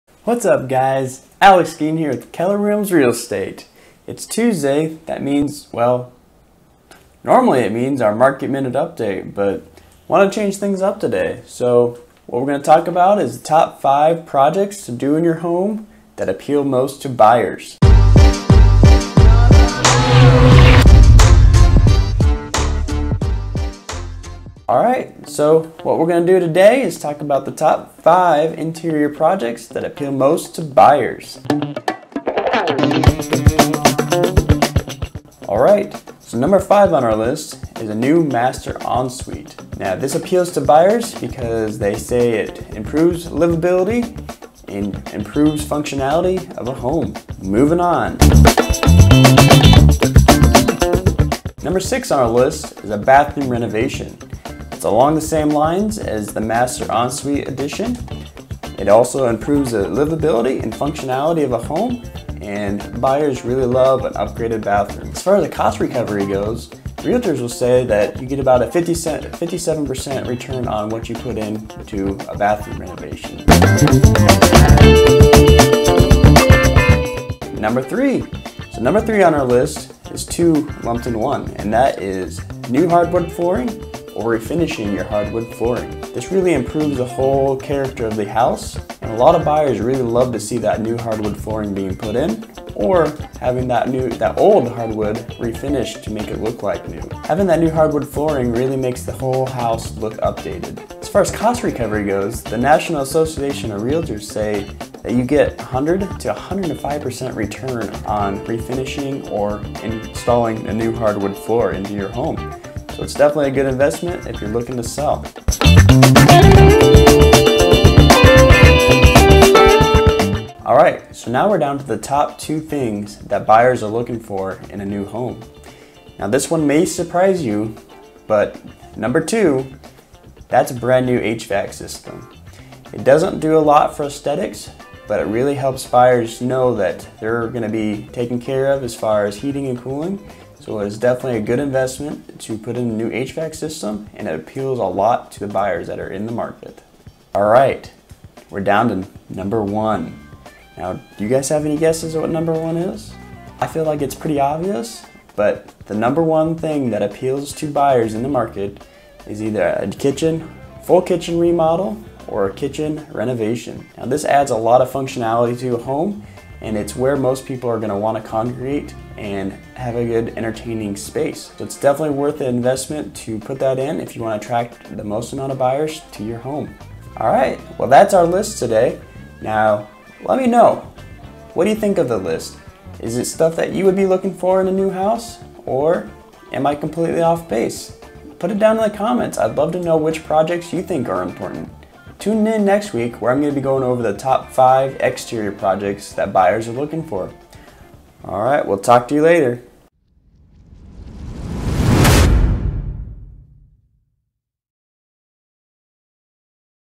Top 5 Interior Home Renovation Projects
As many of you know, if you are interested in this video, that interior home renovation projects are important when selling your home.  In this video I cover the top 5 interior home renovation projects that make sense to do in order to get the best return on your money.

Alex Skeen – Realtor with Keller Williams GQC
Licensed in Iowa & Illinois